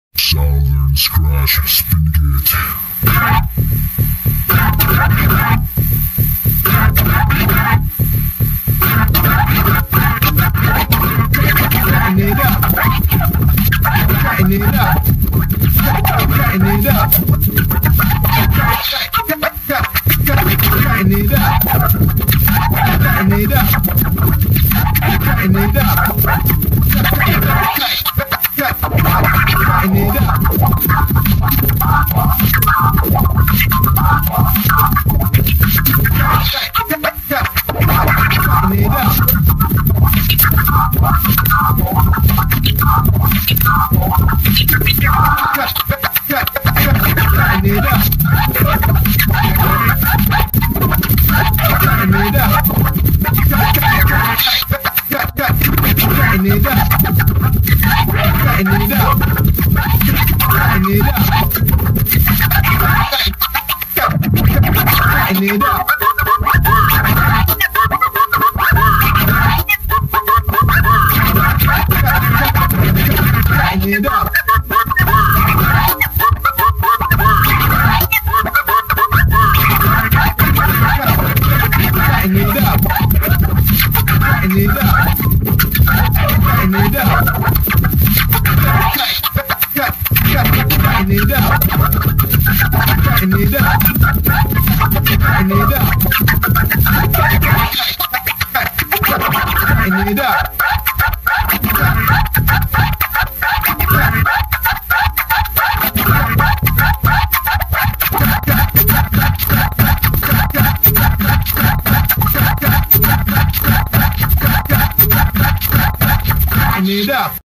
S O U T H E R N S C R A T C H S P I N D K T Spindikato Dyha Radkut
Scratch sessions Southern Scratch Spindkt style using All Turntables

Composed by: Spindikato, Radkut and Dyha
Cebu City Ph.